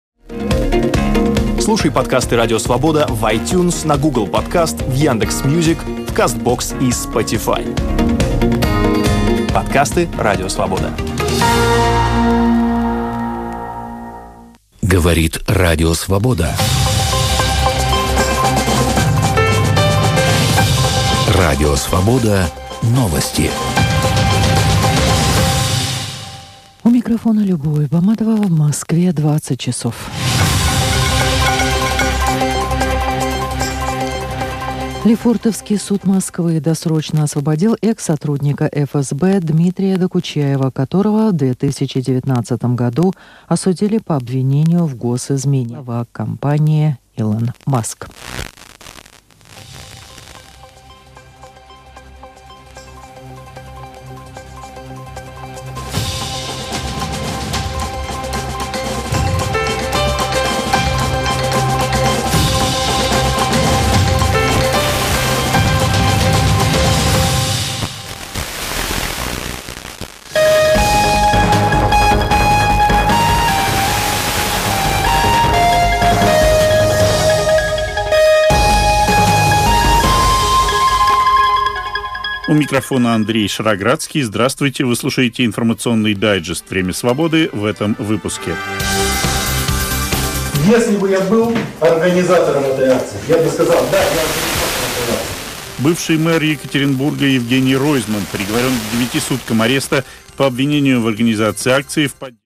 [OIRT FM-DX] Radio Svoboda/Free Europe (Moldova) via Sporadic E in France • 13/05/2021
Reception of Radio Svoboda/Radio Free Europe/Radio Liberty from Strășeni, Moldova via Sporadic E propagation near Lyon, France on May 13, 2021.
Distance: 1,827 kilometers / 1,135 miles

-- Transmitter --
Frequency: 68.48 MHz (OIRT Band)
Location: Strășeni/STRV, Moldova
Power (ERP): 20kW
Polarization: Horizontal

-- Receiver --
Airspy Mini via SDR# software
Antenna: Körner 9.2 - Horizontal Polarization
Location: Brindas, France (10km South-West of Lyon)